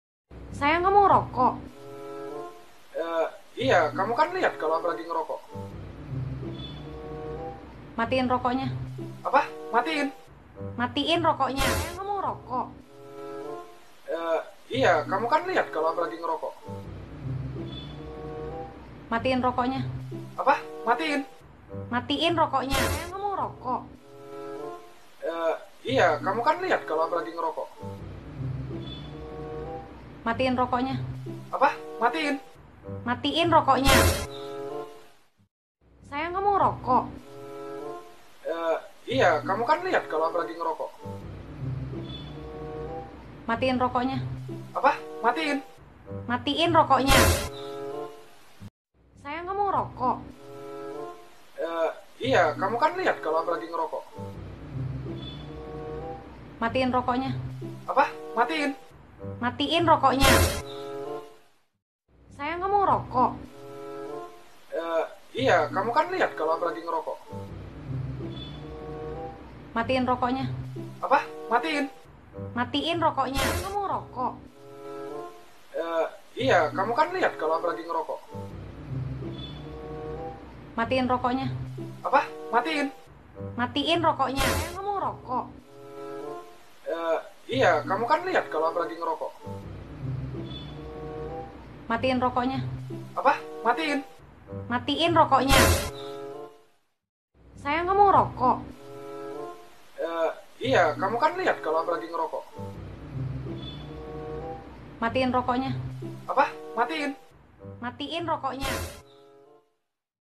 Viral TIKTOK Cewek Cantik Ngelarang Merokok
Apakah Kalian Mau Berhenti Jika Yang Minta Cewek Di Atas Yang Minta

Tulis Jawaban Di Kolom Komentar